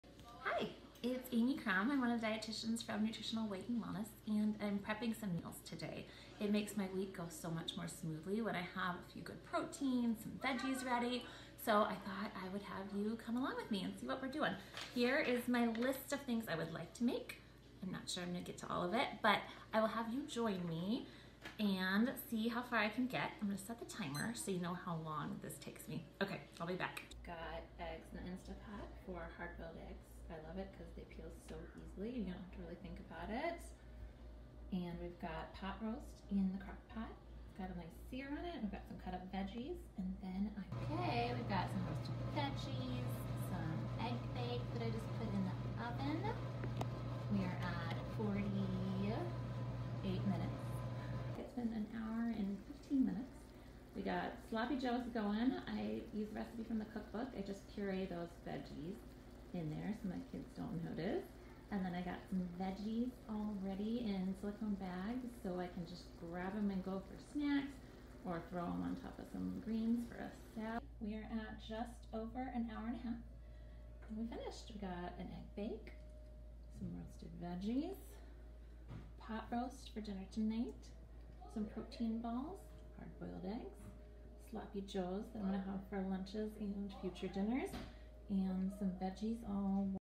Weekend Meal Prep with Amy Crum, RD, LD
Come along with our registered dietitian Amy as she meal preps for the week for her family!

Meal prep doesn't have to be complicated or take a lot of time, but it is a life saver for busy weeks so you always have healthy food on hand in your refrigerator.

What are your favorite things to meal prep?